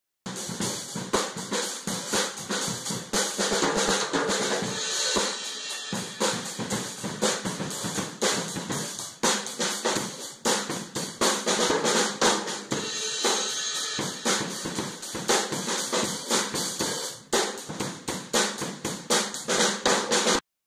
Mark with bass drum variations.
Mark is 15 and has been taking drum lessons with me since age 10.  In that time, he has completed four grades earning two distinctions and two merits.  Mark is currently studying for his grade 6 Rockschool exam and is a keen improviser.